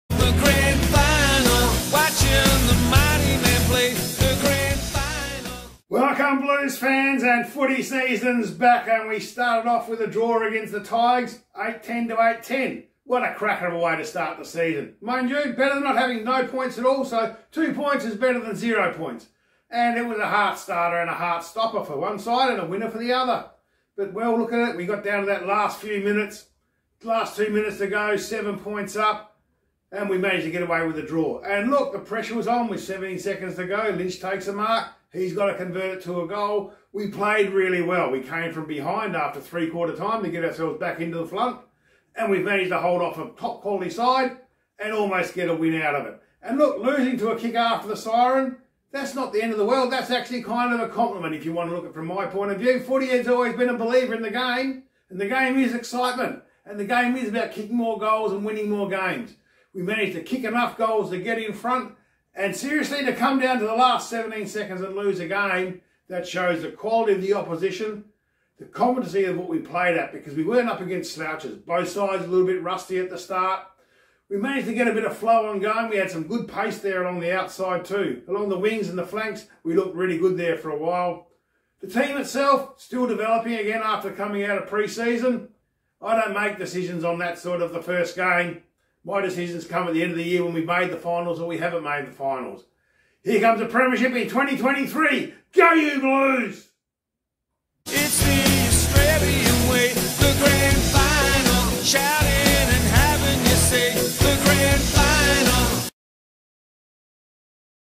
2023 Footyhead Rd 1 V Richmond
Wow, what an intense and exciting match! Carlton and Richmond put up a great fight, and the final score being tied must have been a bit of a let-down for both teams. It's always tough to come so close to a win and then have it snatched away in the final moments.

It's interesting to see the emotions that come into play during a game like this, both for the players and the fans. Tom Lynch was really feeling the pressure after his last-minute elimination final miss, but he was able to keep his focus and make the crucial mark and goal that tied the game. That takes a lot of mental strength and resilience.

Overall, the Blues have started their 2023 footy season. There will be plenty more exciting matches to come. Go you Blues!